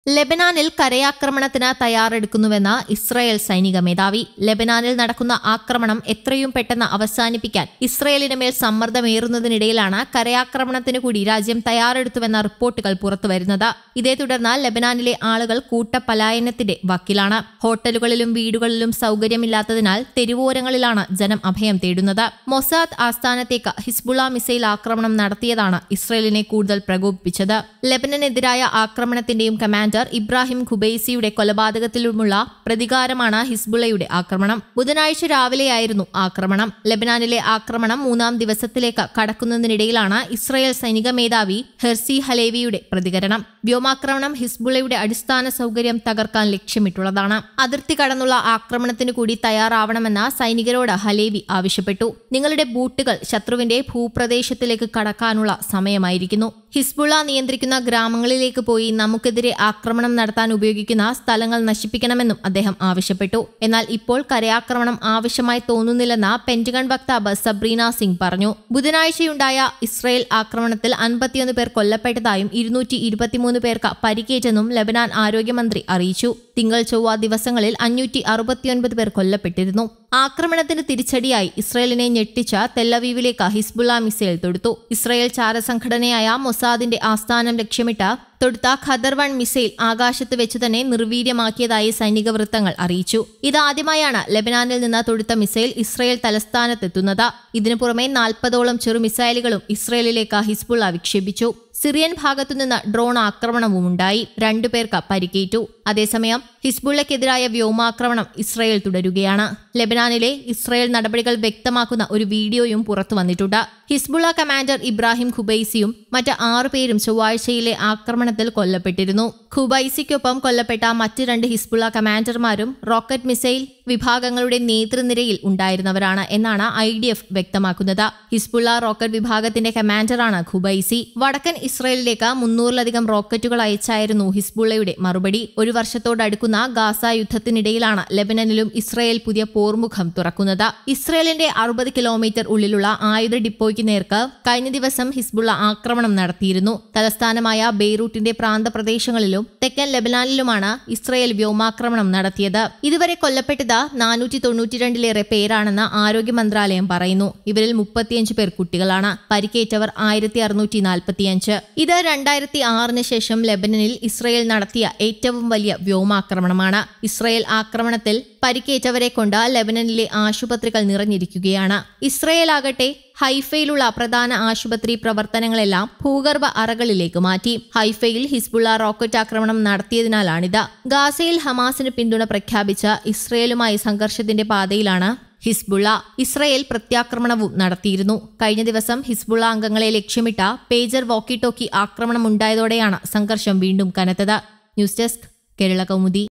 ലബനന്‍ ചിന്നിചിതറും,കരയാക്രമണത്തിന് സജ്ജമായി ഇസ്രയേല്‍ സൈന്യം | Lebanon | Israel Defence Forces
ലബനാനില്‍ കരയാക്രമണത്തിന് തയാറെടുക്കുന്നുവെന്ന് ഇസ്രായേല്‍ സൈനിക മേധാവി. ലബനാനില്‍ നടക്കുന്ന ആക്രമണം എത്രയും പെട്ടെന്ന് അവസാനിപ്പിക്കാന്‍ ഇസ്രായേലിന് മേല്‍ സമ്മര്‍ദമേറുന്നതിനിടെയാണ് കരയാക്രമണത്തിന് കൂടി രാജ്യം തയാറെടുത്തുവെന്ന റിപ്പോര്‍ട്ടുകള്‍ പുറത്ത് വരുന്നത്. ഹോട്ടലുകളിലും വീടുകളിലും സൗകര്യമില്ലാത്തതിനാല്‍ തെരുവോരങ്ങളിലാണു ജനം അഭയം തേടിയത്. മൊസാദ് ആസ്ഥാനത്തേയ്ക്ക് ഹിസ്ബുള്ള മിസൈല്‍ ആക്രമണം നടത്തിയതാണ് ഇസ്രയേലിനെ കൂടുതല്‍ പ്രകോപിപ്പിച്ചത്. ലെബനനെതിരായ ആക്രമണത്തിന്റേയും കമാന്‍ഡര്‍ ഇബ്രാഹിം ഖുബൈസിയുടെ കൊലപാതകത്തിലുമുള്ള പ്രതികാരമാണു ഹിസ്ബുള്ളയുടെ ആക്രമണം. ബുധനാഴ്ച രാവിലെയായിരുന്നു ആക്രമണം. ലബനാനിലെ ആക്രമണം മൂന്നാം ദിവസത്തിലേക്ക് കടക്കുന്നതിനിടെയാണ് ഇസ്രായേല്‍ സൈനിക മേധാവി ഹെര്‍സി ഹാലേവിയുടെ പ്രതികരണം. വ്യോമാക്രമണം ഹിസ്ബുല്ലയുടെ അടിസ്ഥാന സൗകര്യം തകര്‍ക്കാന്‍ ലക്ഷ്യമിട്ടുള്ളതാണ്. അതിര്‍ത്തി കടന്നുള്ള ആക്രമണത്തിന് കൂടി തയാറാവണമെന്ന് സൈനികരോട് ഹാലേവി ആവശ്യപ്പെട്ടു. നിങ്ങളുടെ ബൂട്ടുകള്‍ ശത്രുവിന്റെ ഭൂപ്രദേശത്തിലേക്ക് കടക്കാനുള്ള സമയമായിരിക്കുന്നു. ഹിസ്ബുല്ല നിയന്ത്രിക്കുന്ന ഗ്രാമങ്ങളിലേക്ക് പോയി, നമുക്കെതിരെ ആക്രമണം നടത്താന്‍ ഉപയോഗിക്കുന്ന സ്ഥലങ്ങള്‍ നശിപ്പിക്കണമെന്നും അദ്ദേഹം ആവശ്യപ്പെട്ടു. എന്നാല്‍, ഇപ്പോള്‍ കരയാക്രമണം ആവശ്യമായി തോന്നുന്നില്ലെന്ന് പെന്റഗണ്‍ വക്താവ് സബ്രീന സിങ് പറഞ്ഞു.